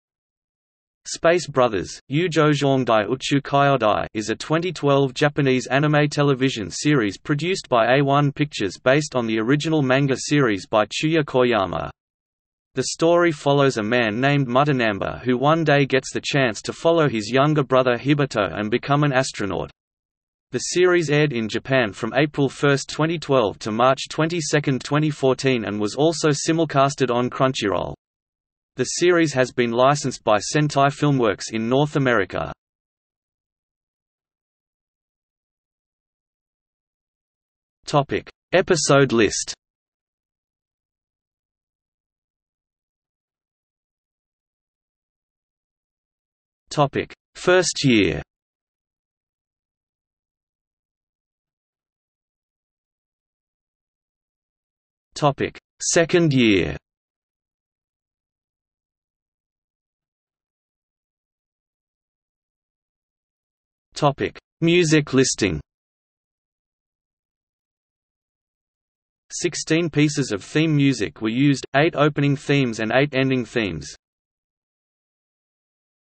List of Space Brothers episodes | Wikipedia audio article
00:00:34 1 Episode list
00:00:43 1.1 1st year
00:00:51 1.2 2nd year
00:01:00 2 Music listing

- Socrates

SUMMARY
Space Brothers (宇宙兄弟, Uchū Kyōdai) is a 2012 Japanese anime television series produced by A-1 Pictures based on the original manga series by Chūya Koyama. The story follows a man named Mutta Nanba who one day gets the chance to follow his younger brother Hibito and become an astronaut. The series aired in Japan from April 1, 2012 to March 22, 2014 and was also simulcasted on Crunchyroll. The series has been licensed by Sentai Filmworks in North America.